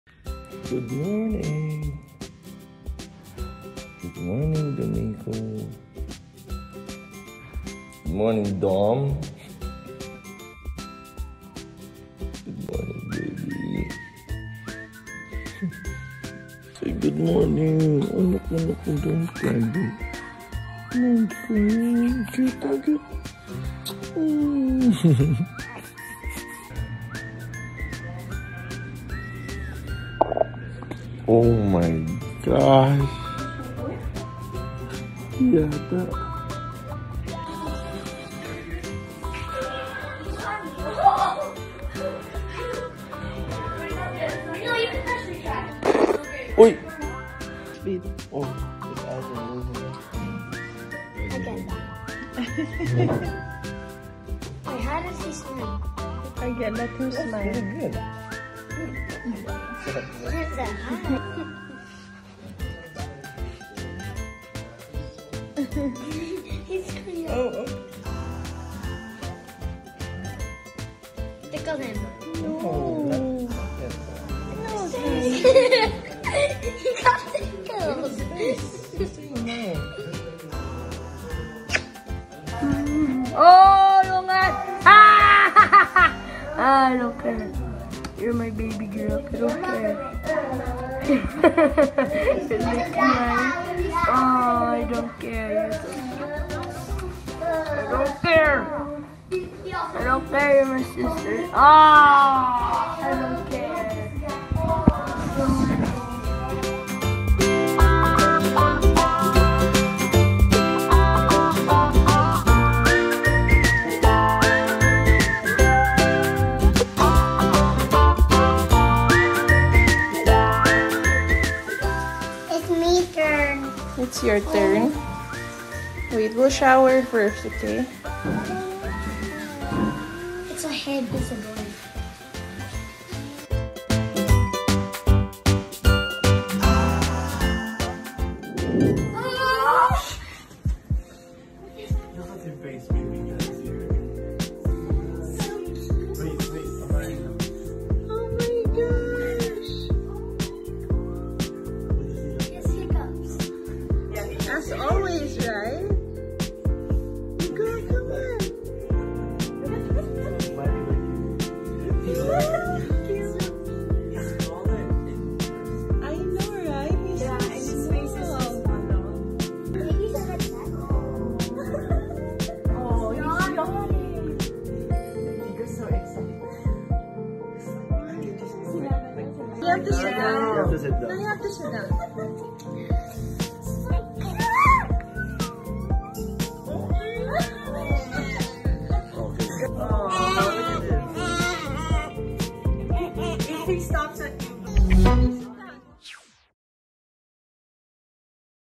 Embarrassing moment for daddy | KAMI | Every time Gian Sotto holds his newborn son
Every time Gian Sotto holds his newborn son, the baby produces an unforgettable sound that makes the Quezon City Vice Mayor feel astonished